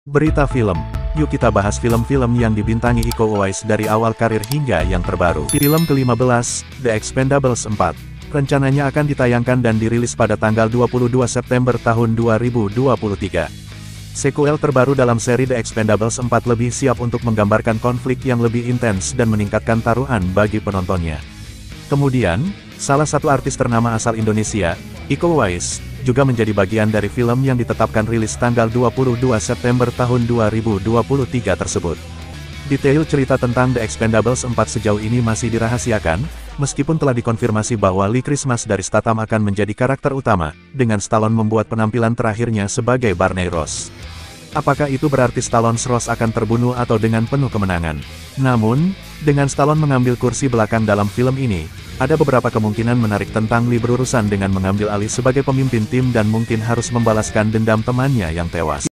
Film Iko Uwais 2023 Mencekik Jason Statham| The Expendables 4
berita film dari Iko Uwais.

kali ini akan membahas film2 yang di bintangi iko uwais. siapa yang tidak kenal iko uwais? aktor tampan yang mulai terkenal di jagat perfilman dunia melalui film the raid.namanya mulai hadir di film2 hollywood. yuk kita bahas film2 yg di bintangi iko uwais dari awal karir hingga yang terbaru.
film terbarunya malah menjadi musuh bebuyutan di expendables 4, iko bakalan beradu jotos dengan jason statham.

film ke 15. the expendables 4
rencananya akan ditayangkan dan di Rilis pada 22 September 2023.
Sekuel terbaru dalam seri The Expendables 4 lebih siap untuk menggambarkan konflik yang lebih intens dan meningkatkan taruhan bagi penontonnya. Kemudian, salah satu artis ternama asal Indonesia, Iko Uwais, juga menjadi bagian dari film yang ditetapkan rilis 22 September 2023 tersebut.

Dipersenjatai dengan setiap senjata yang bisa mereka dapatkan, Expendables adalah garis pertahanan terakhir dunia dan tim yang dipanggil ketika semua opsi lain tidak tersedia.

Detail cerita tentang The Expendables 4 sejauh ini masih dirahasiakan, meskipun telah dikonfirmasi bahwa Lee Christmas dari Statham akan menjadi karakter utama, dengan Stallone membuat penampilan terakhirnya sebagai Barney Ross. Apakah itu berarti Stallone's Ross akan terbunuh atau dengan penuh kemenangan. Namun, dengan Stallone mengambil kursi belakang dalam film ini, ada beberapa kemungkinan menarik tentang Lee berurusan dengan mengambil alih sebagai pemimpin tim dan mungkin harus membalaskan dendam temannya yang tewas.